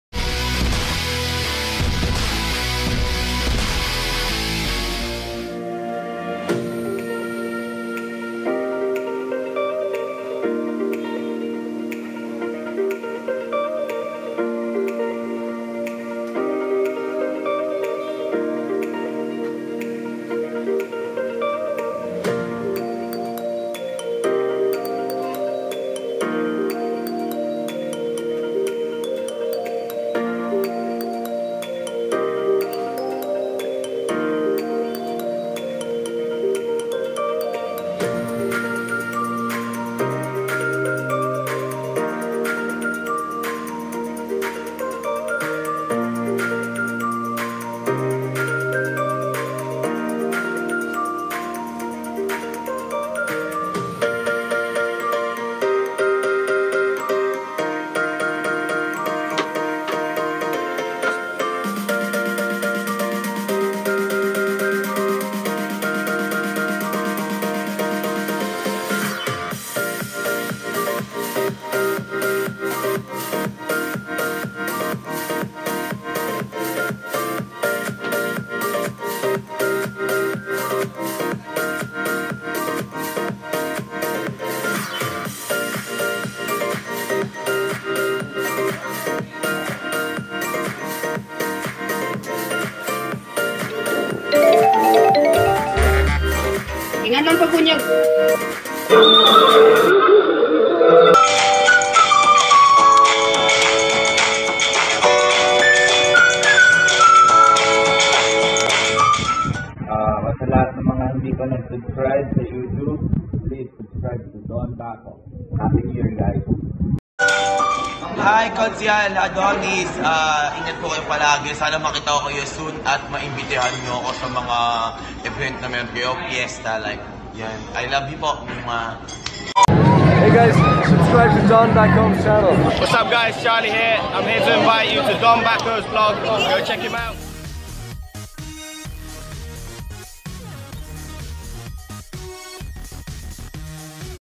Sulfur Vents -valencia,dumaguete city
it’s like a “Smokey Mountain”. It is just around 30mins or less ride from Dumaguete proper.The smokes and gases are like rotten eggs or the smell of a C.R. It is a natural toxic gas coming from the walls or stones of the area as it turns out Valencia has a lot of these geothermal vents spewing sulfuric smoke. There is a geothermal plant nearby and is worth a visit if you have time.